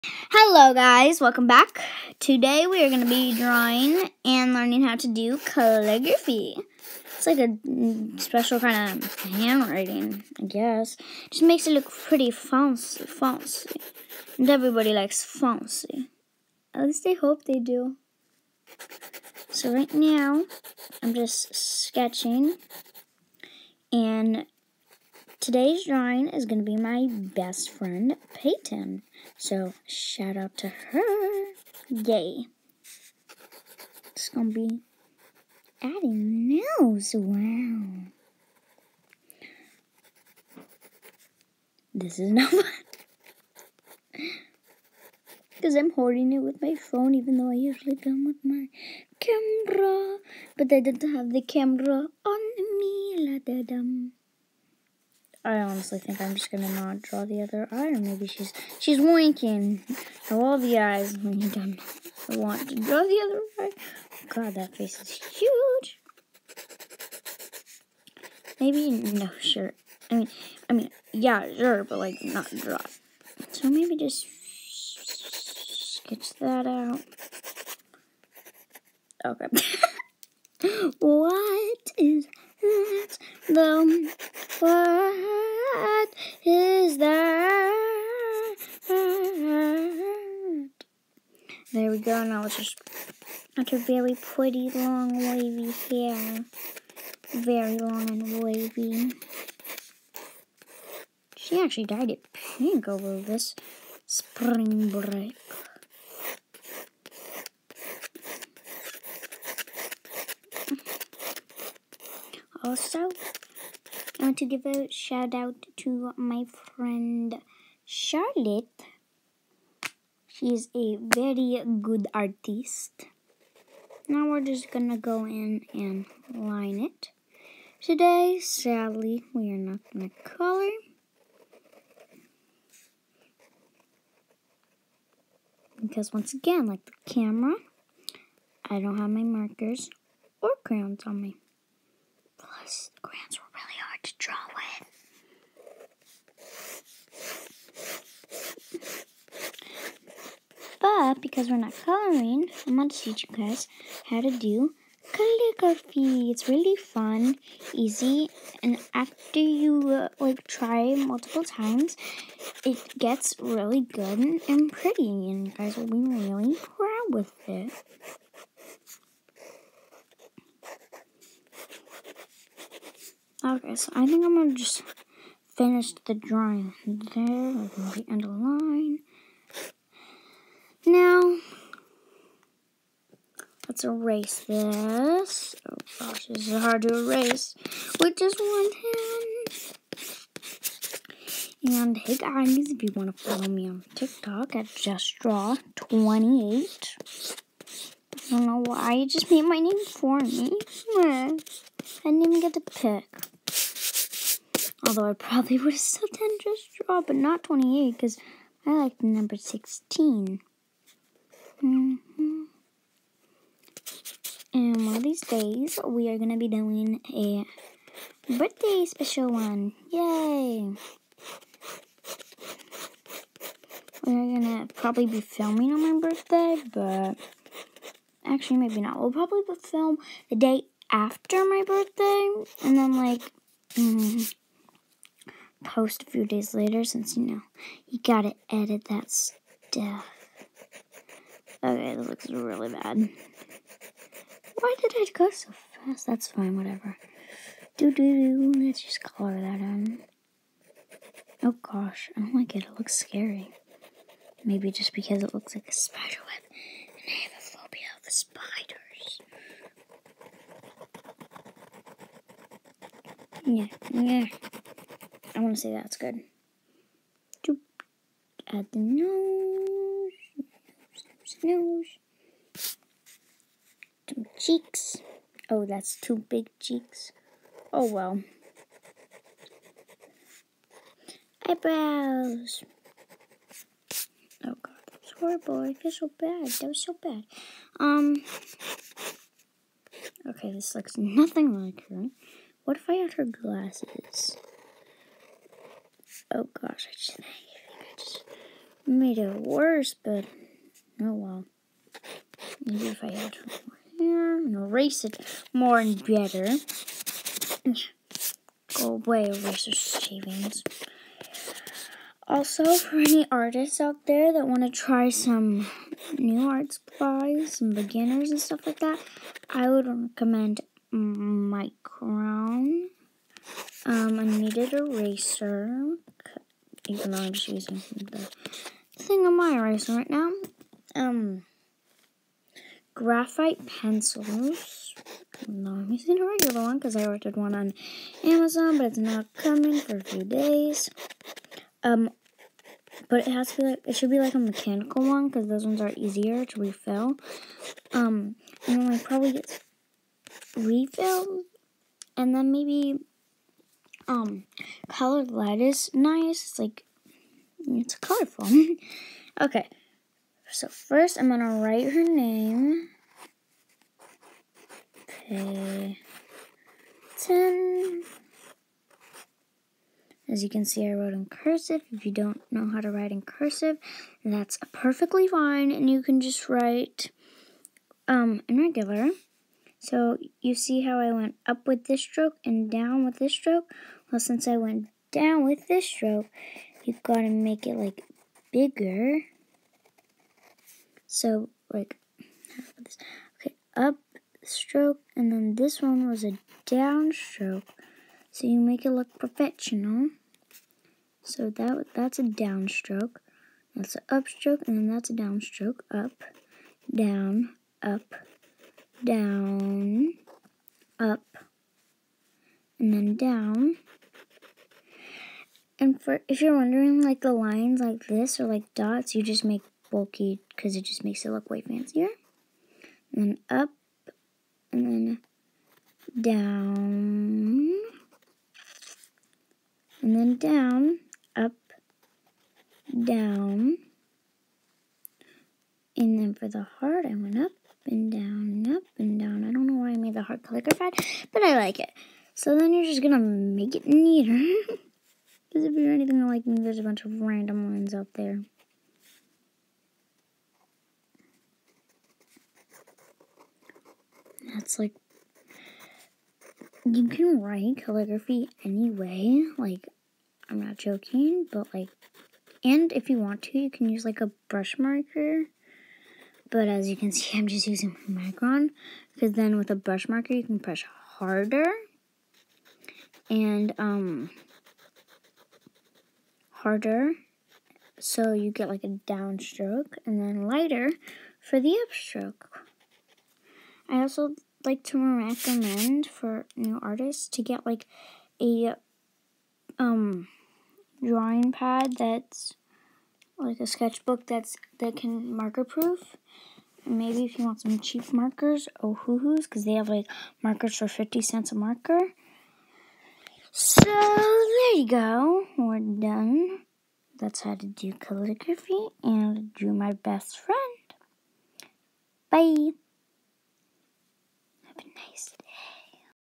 HOW TO DO CALLIGRAPHY!?!
Today we are going to be drawing my best friend Payton and learning how to do calligraphy.